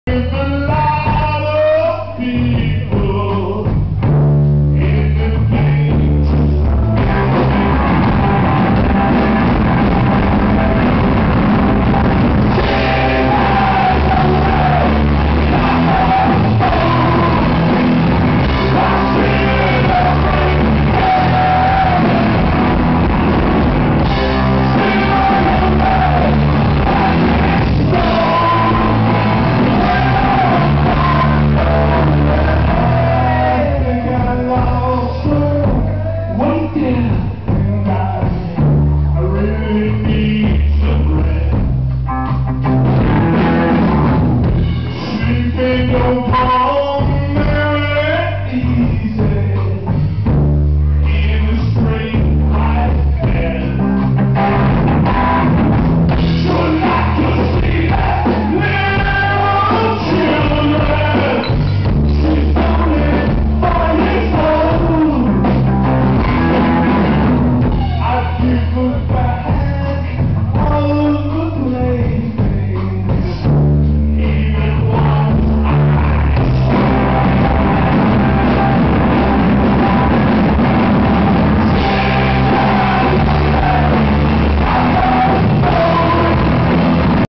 the melvins @ the troubadour 7-27-07
the melvins.  taken with digital cam.  good show. lo fi vid ;]